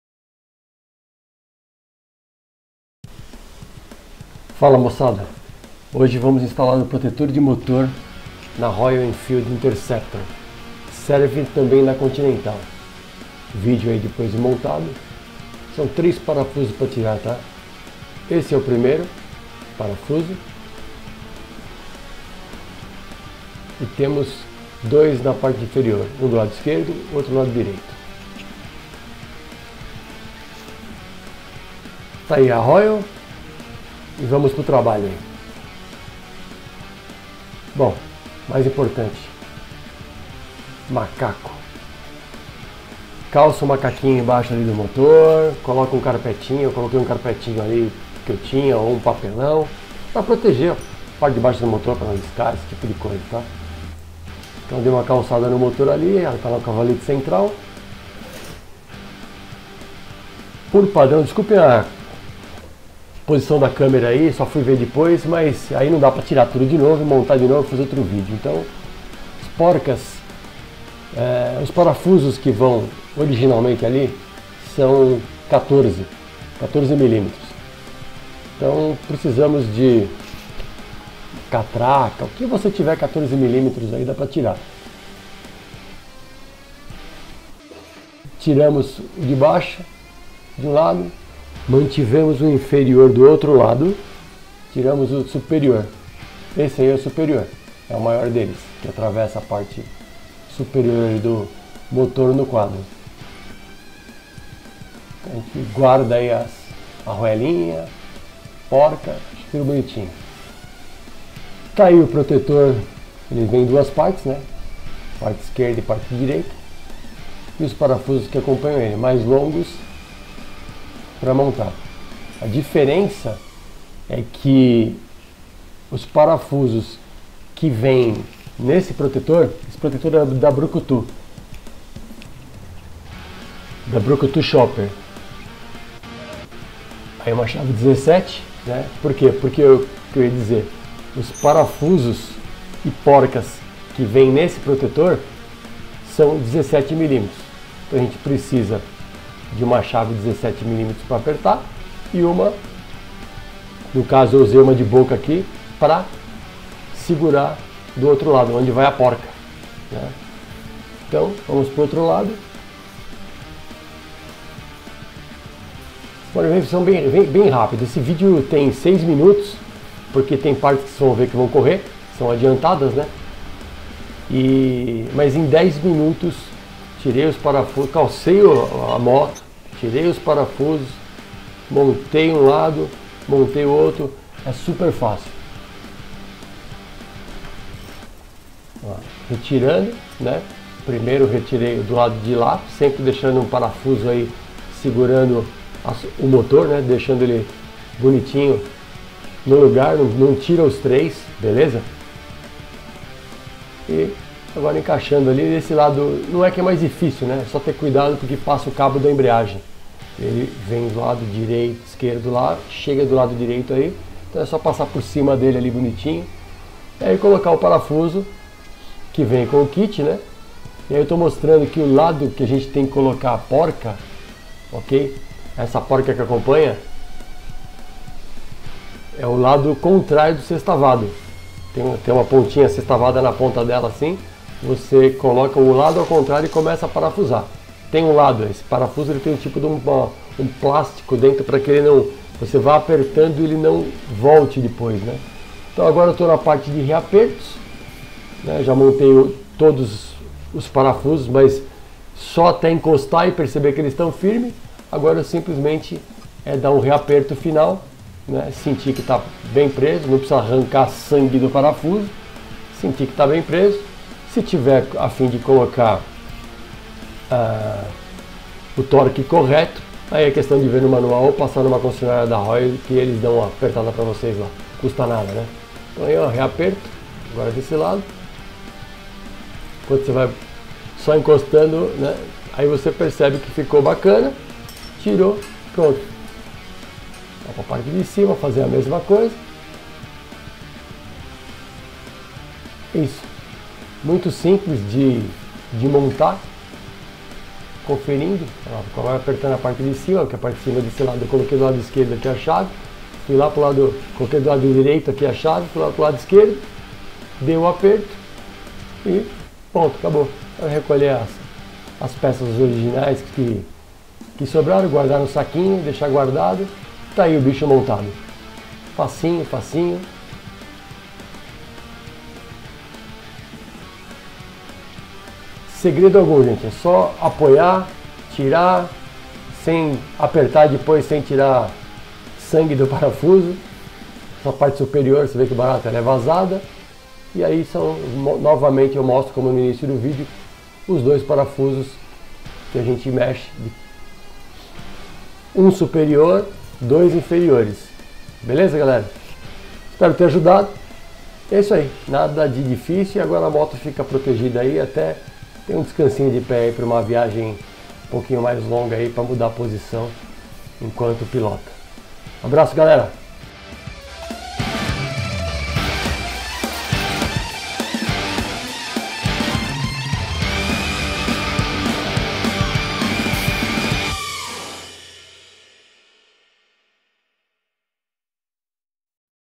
Montagem do Protetor de Motor Interceptor 650 ou Continental GT 650 - Engine Guard
Este é da marca BRUKUTU CHOPPERS, os quais tem outros acessórios para as Royal Enfield, bem como outras marcas de motos customs

A montagem é bem simples e neste tutorial mostro que é possível instalar você mesmo em 10 minutos